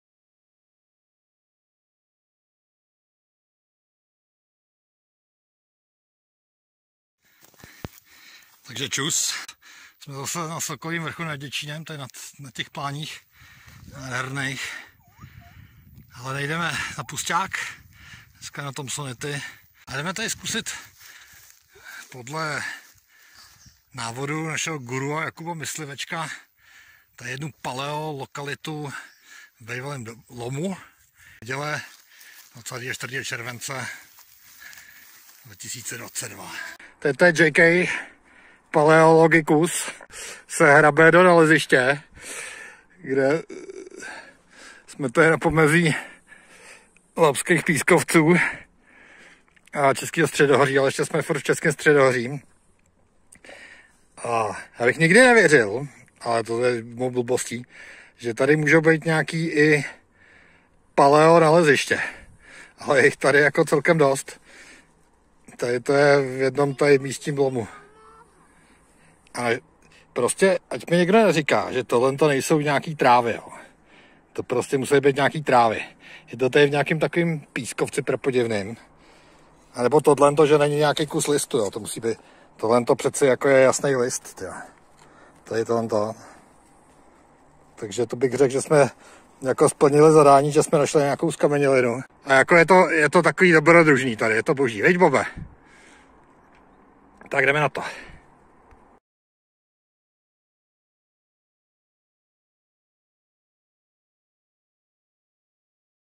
Stones, se01ep06: JayKay a první paleo u Ludvíkovic/Huntířova, 24.7.2022
Stones, se01ep06: JayKay and 1st paleo close to Ludvíkovice/Huntířov, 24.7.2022

Asi možná Stones, se01ep05, byť teda v paleu. Ale zábavka dobrá, dobrodrůžo taky, nějaký vobtisklý listí jsme našli.